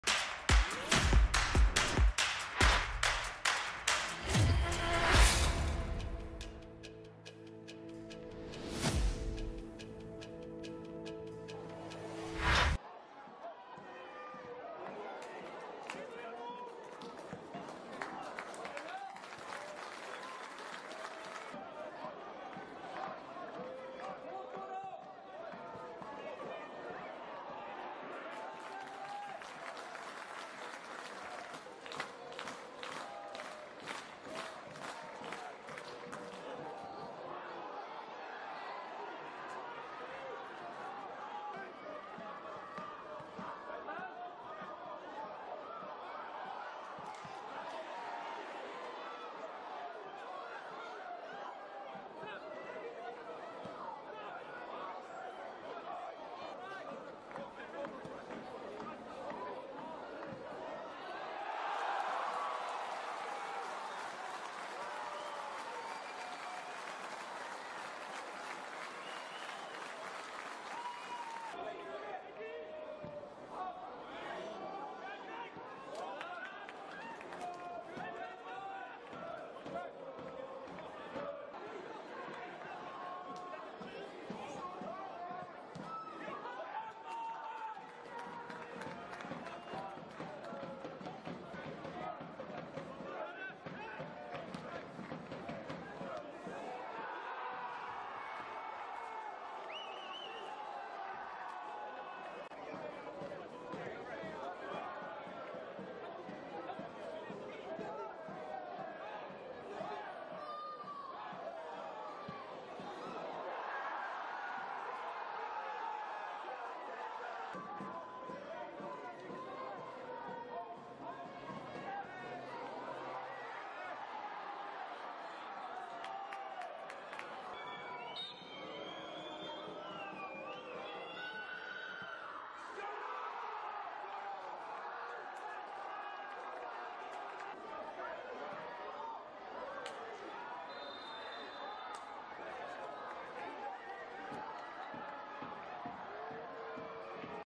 Höjdpunkter: Falkenbergs FF - Ljungskile SK - 2023-05-01
Höjdpunkter från matchen, i Ettan Södra, Falkenberg - LSK som slutade 1-3 (1-0).

Målskyttar LSK:
Jared Odenbeck
JT Kamara
Aulon Bitiqi